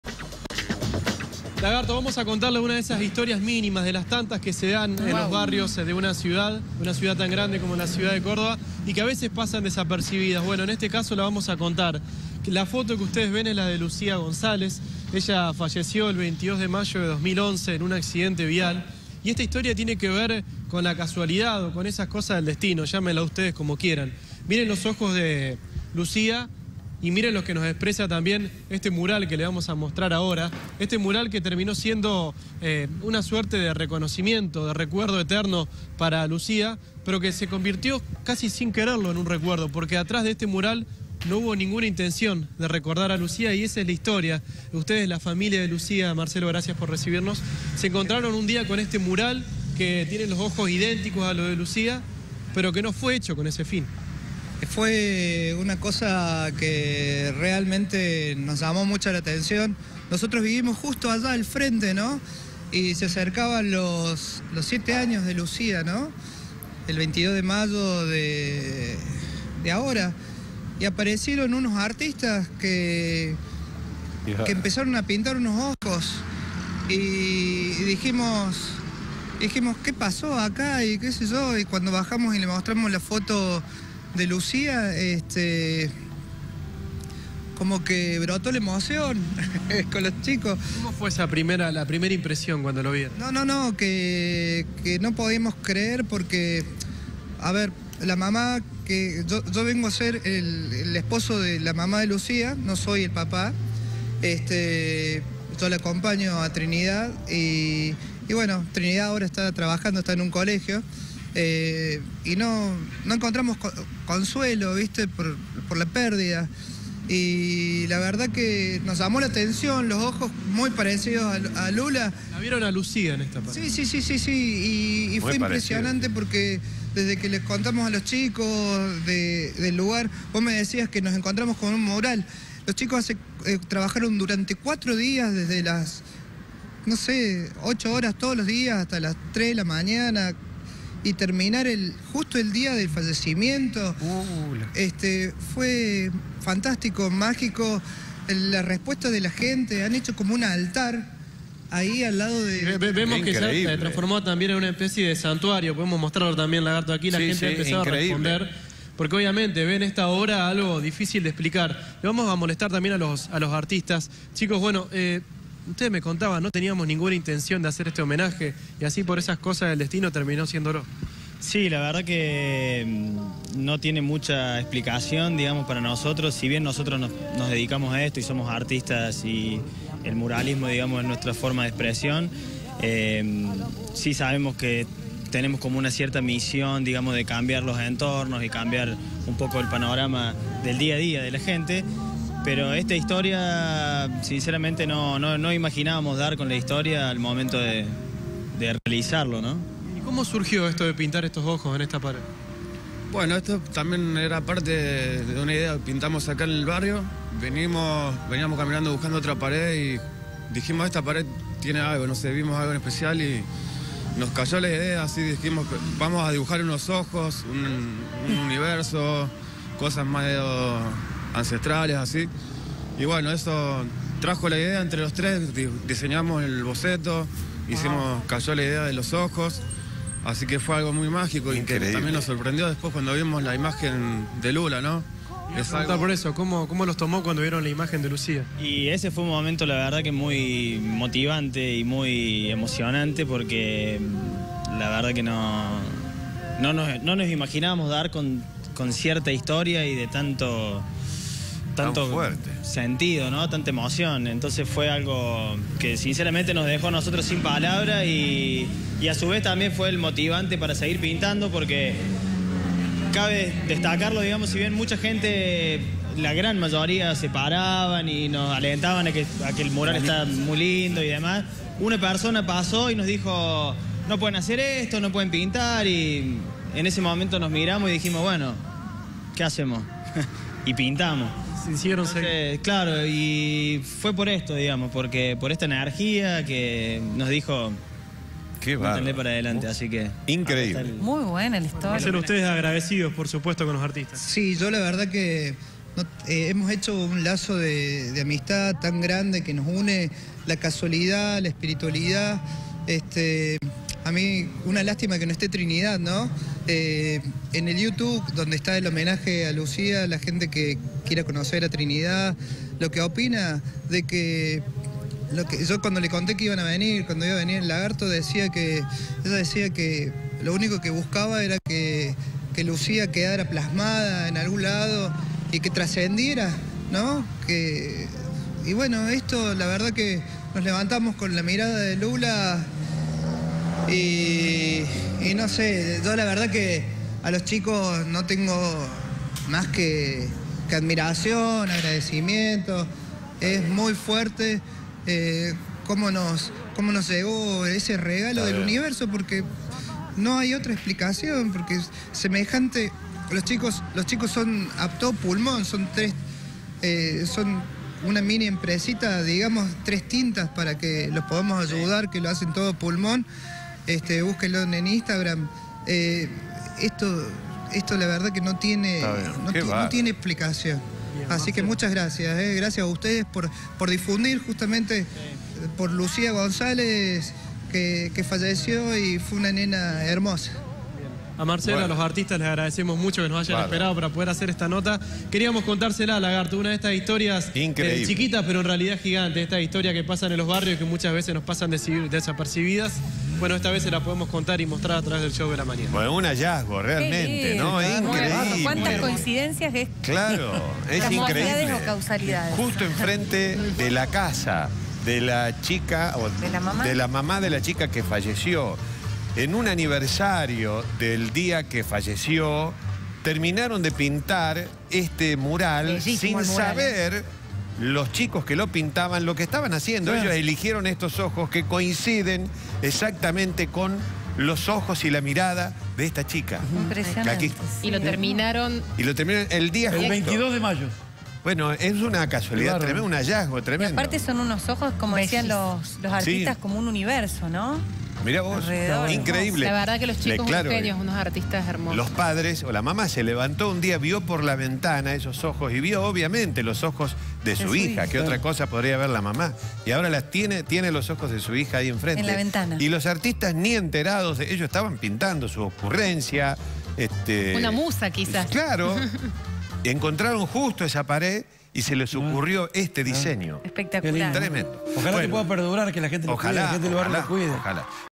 Homenajearon a una menor muerta sin saberlo
Esas cosas del destino hicieron coincidir a un mural con el recuerdo de los ojos de una niña de 10 años que falleció. Sin saberlo, los muralistas pintaron una mirada que recuerda a "lula", una chica que vivía en la casa del frente.

Marcelo Cafure, padrastro de Lucía, relata la increíble historia. En el lugar se instaló un pequeño santuario. Los artistas Ricardo Denef y Pablo Porcel nos cuentan cómo surgió la idea y la sorpresa que se llevaron cuando conocieron las coincidencias.